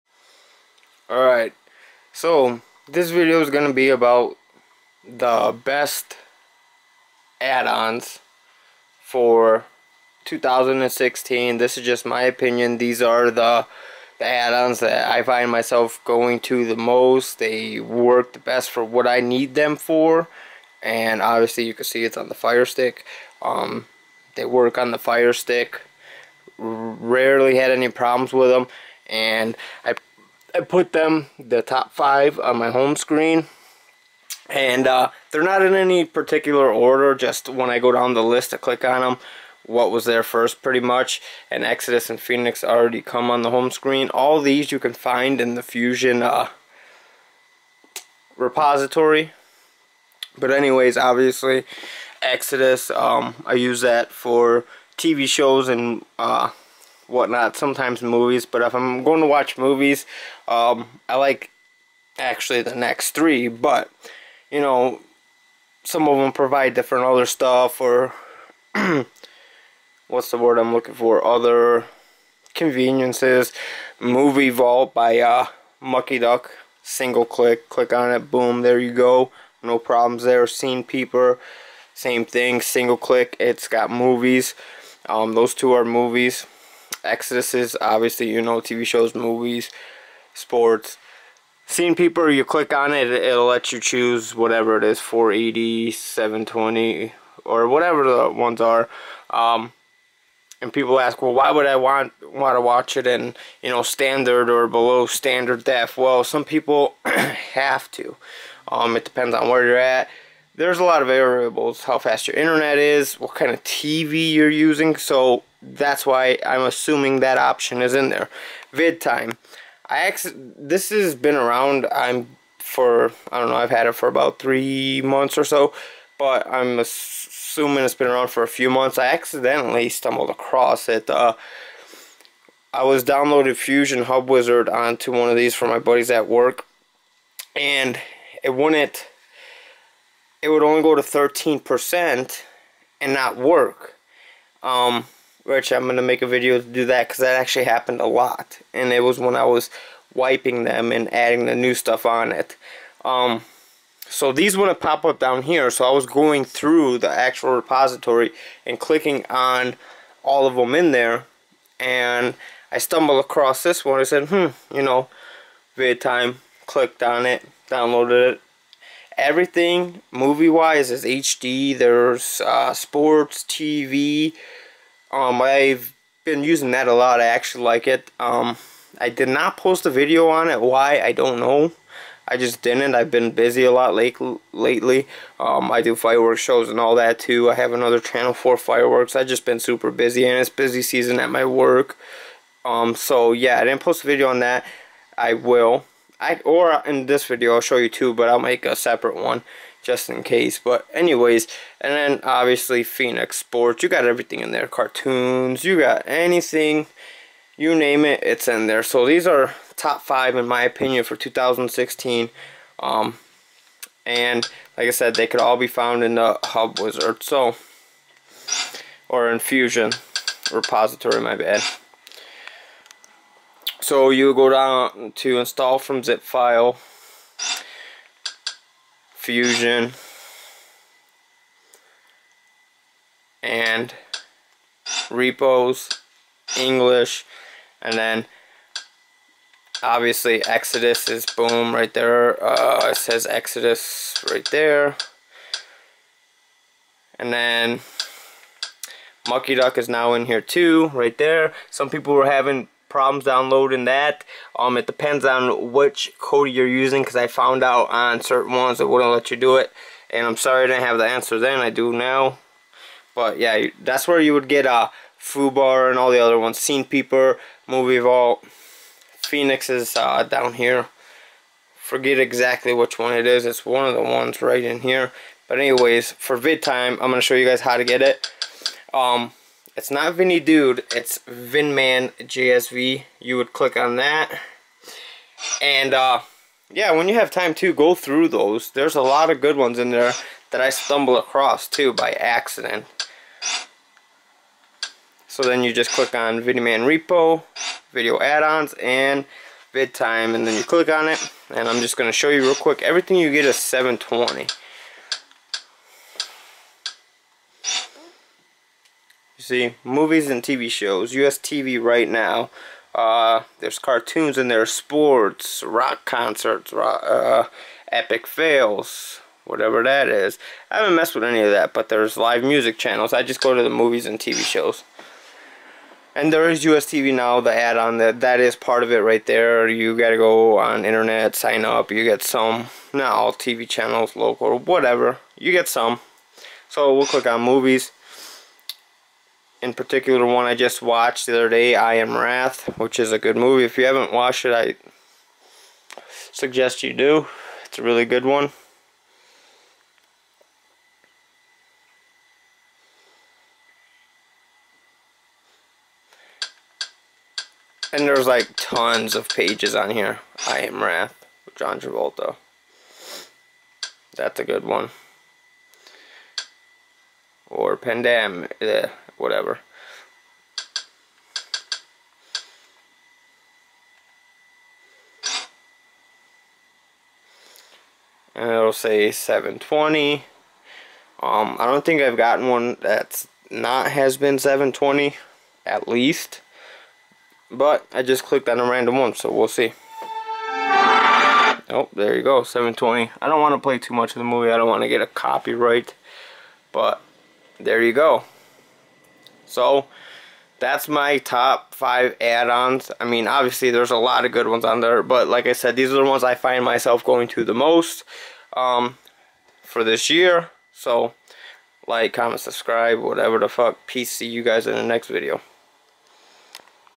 THE BEST KODI ADDONS 2016!!!
THESE ARE THE BEST IN MY OPINION SO FAR THIS YEAR.

I am sharing my knowledge on this subject what you choose to do with it is on you. These are third party addons kodi has nothing to do with these addons so if any problems dont go to kodi go to the addons info and it will show you where to go if any issues.